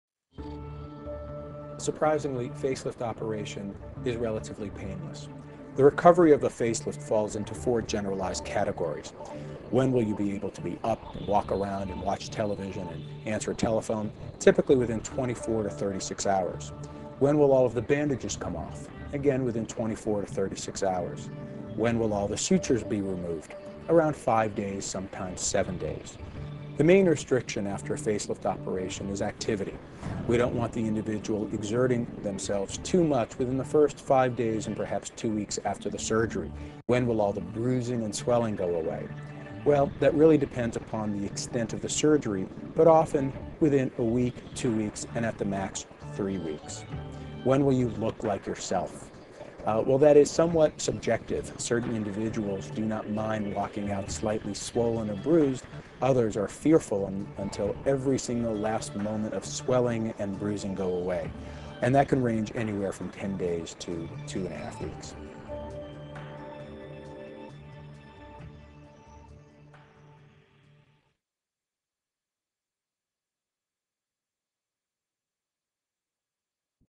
Recovery from Face Lift Surgery
In this video, Dr. Philip Miller discusses the recovery period from face lift surgery that patients experience post operation.